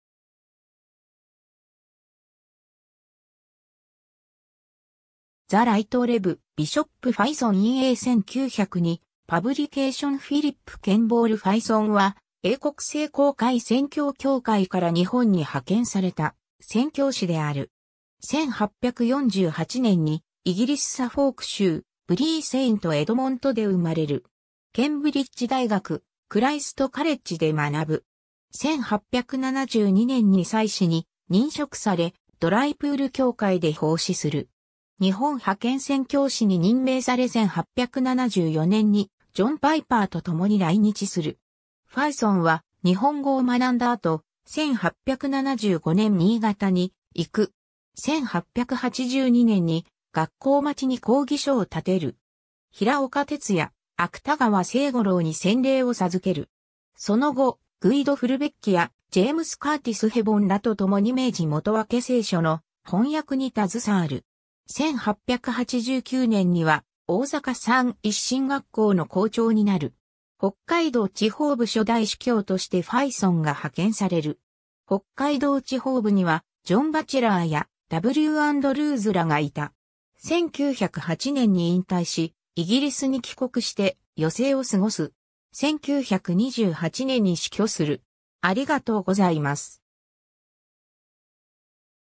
フィリップ・ファイソン
"The Right Rev. Bishop Fyson" in a 1902 publication フィリップ・ケンボール・ファイソン（Philip Kemball Fyson、1846年 - 1928年1月30日）は、英国聖公会宣教協会(CMS)から日本に派遣された宣教師である。
 1848年にイギリスサフォーク州ブリー・セイント・エドモントで生まれる。
ケンブリッジ大学クライスト・カレッジで学ぶ。
1872年に祭司に任職され、ドライプール教会で奉仕する。
 日本派遣宣教師に任命され1874年(明治6年)にジョン・パイパーと共に来日する。
ファイソンは、日本語を学んだ後、1875年新潟に行く。
1882年に学校町に講義所を建てる。
牧岡鉄弥、芥川清五郎に洗礼を授ける。
 その後、グイド・フルベッキやジェームス・カーティス・ヘボンらと共に明治元訳聖書の翻訳に携わる。
 1889年には大阪三一神学校の校長になる。
北海道地方部初代主教としてファイソンが派遣される。
北海道地方部には、ジョン・バチェラーやW・アンドルーズらがいた。
 1908年に引退し、イギリスに帰国して余生を過ごす。
1928年に死去する。